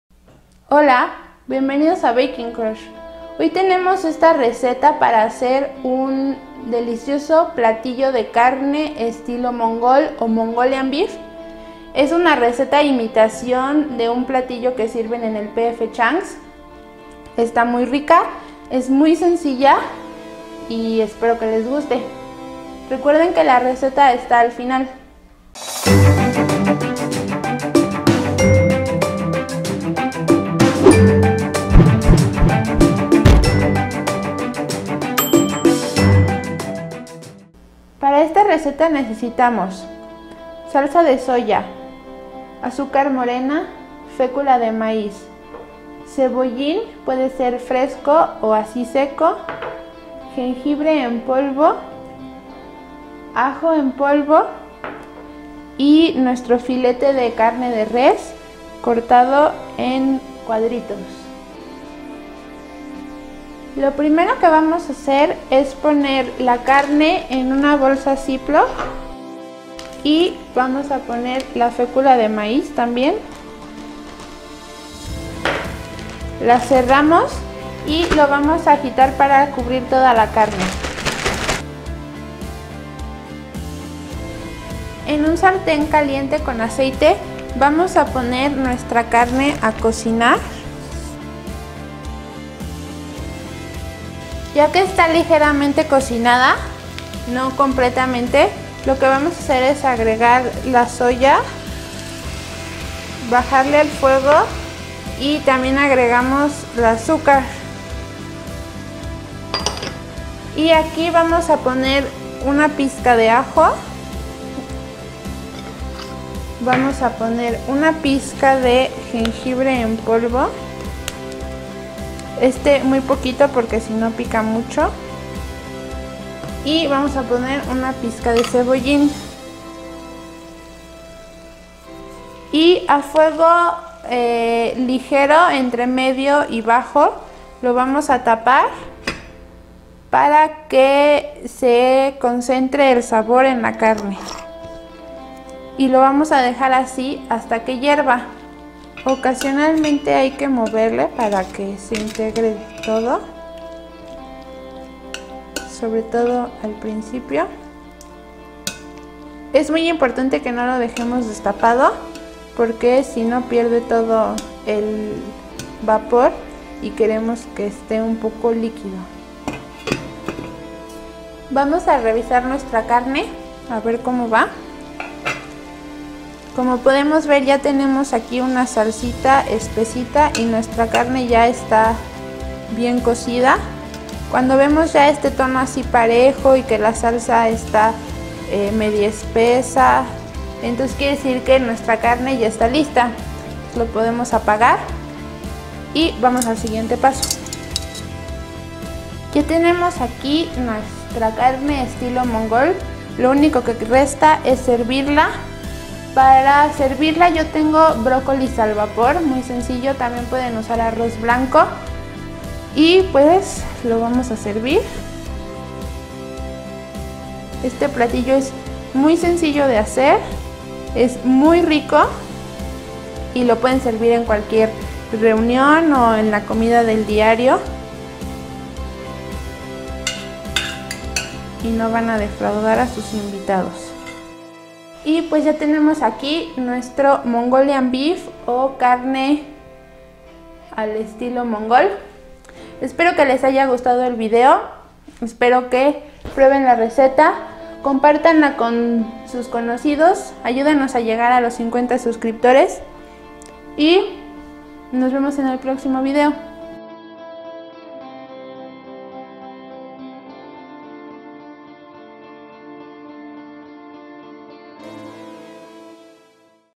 PF Chang’s Mongolian Beef Copycat
Esta receta imita un platillo del restaurante P.F. Chang´s (comida asiática) el cual deleitará a todos tus invitados; es sencillo y rápido.